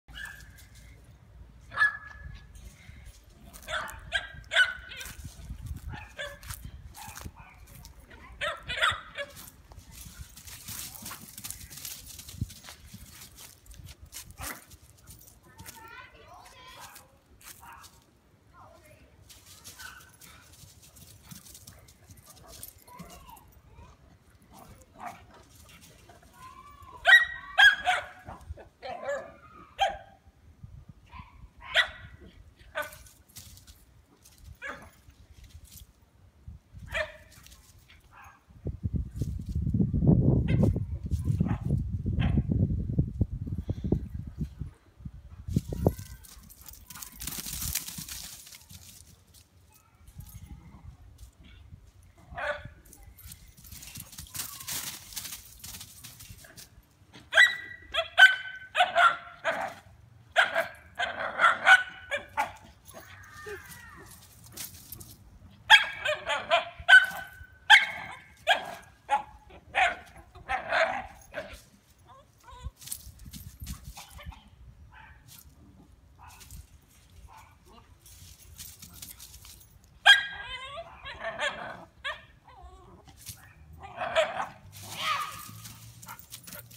Lainie protects the yard!
I don't know why but Lainie's in a very fiest mood this weekend!